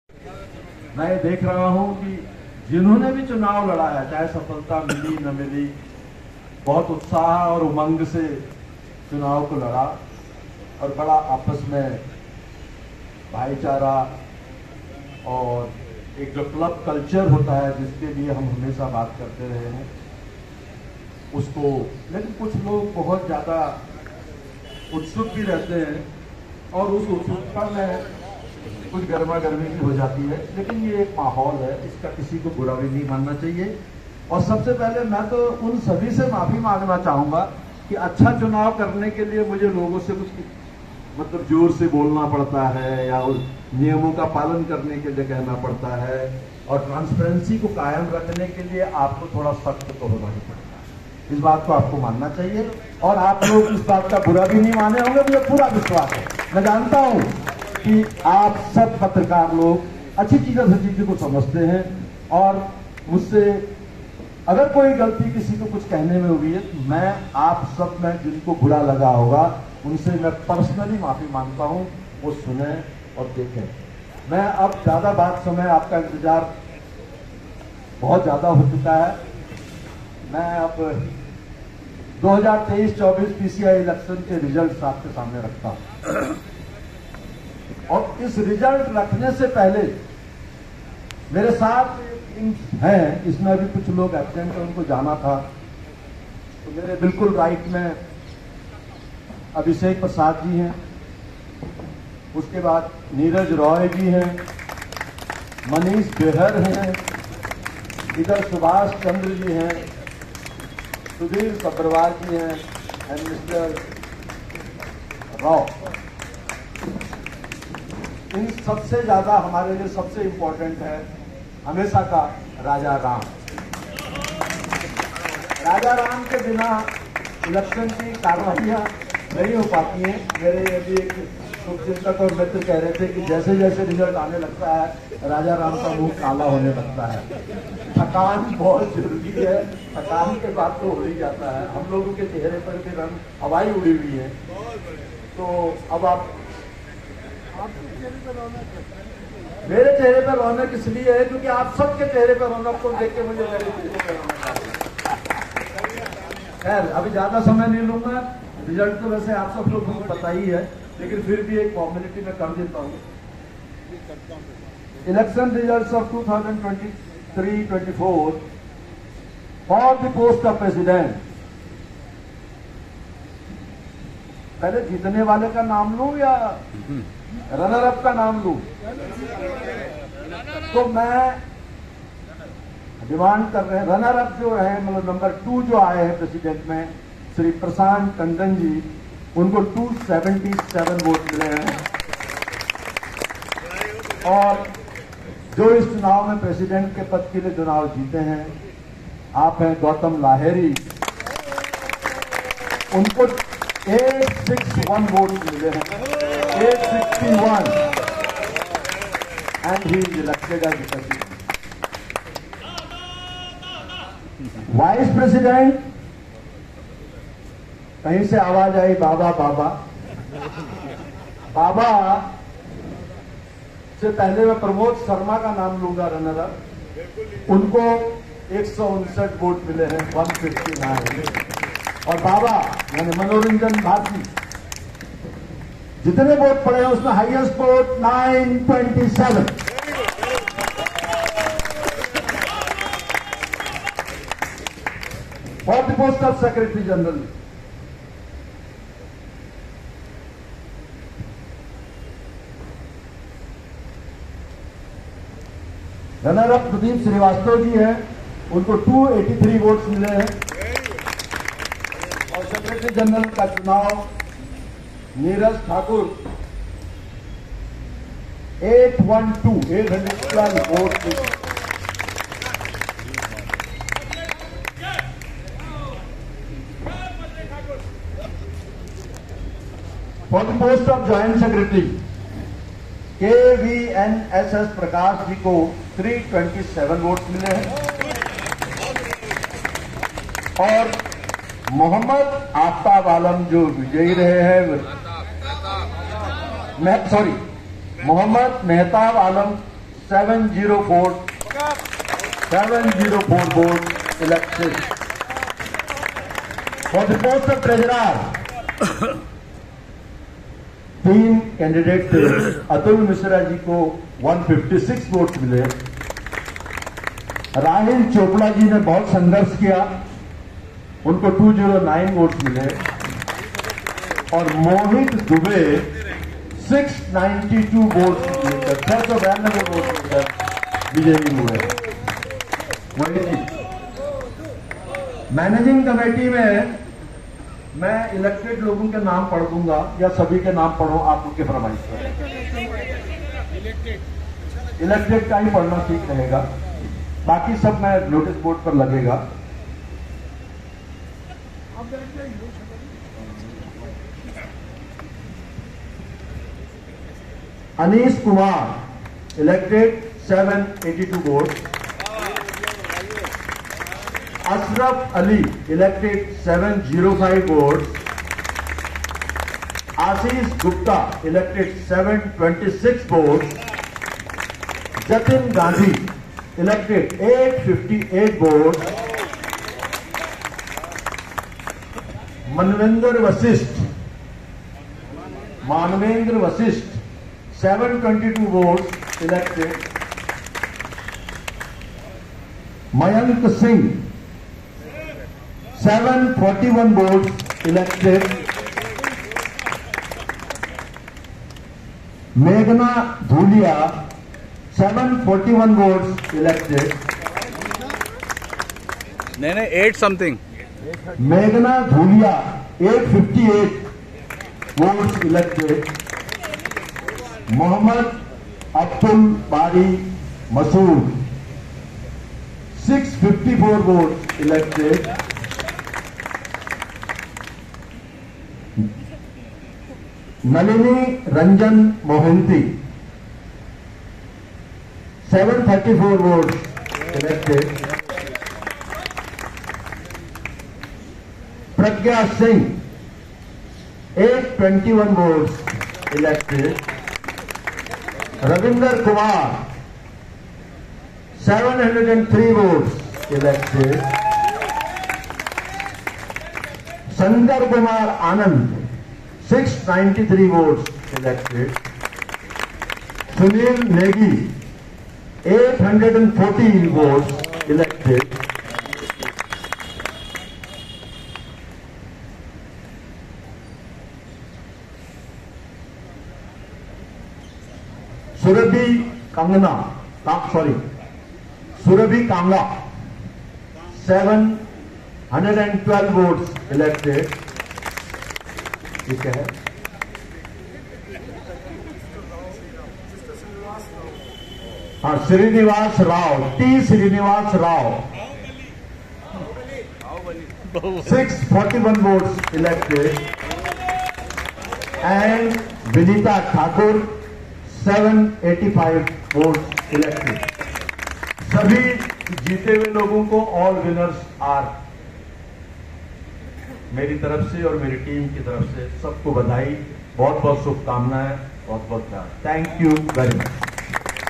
PCI election commissioner declares results of Press Club. Gautam Lehri Panel sweeps the polls.
The panel of Gautam Lahri has won the election of Press Club of India by a very good margin sweeping the polls winning on all seats. It's indeed a grand victory. Gracious thanks to all the valued members of PCI for their unwavering support and ofcource to newly elected President, PCI Gautam Lahiri and his entire team of committed and dedicated journalists.